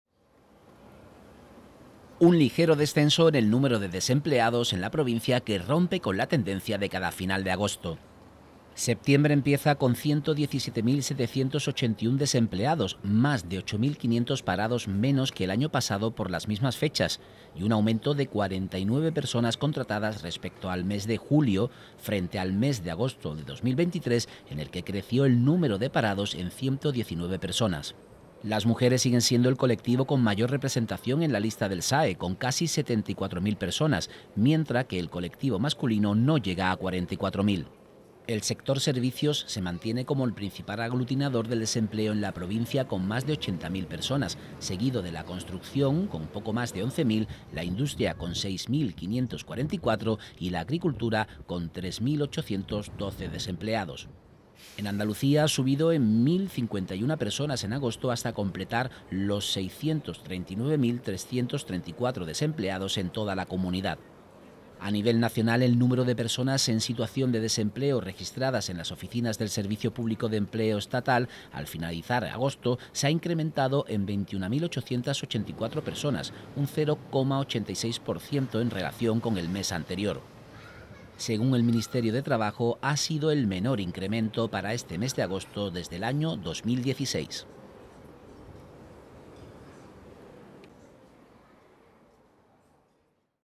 Leve descenso del desempleo en la provincia de Cádiz en agosto
El Ministerio de Trabajo y Economía Social ha publicado los datos del paro correspondiente a un mes que el saldo habitualmente es positivo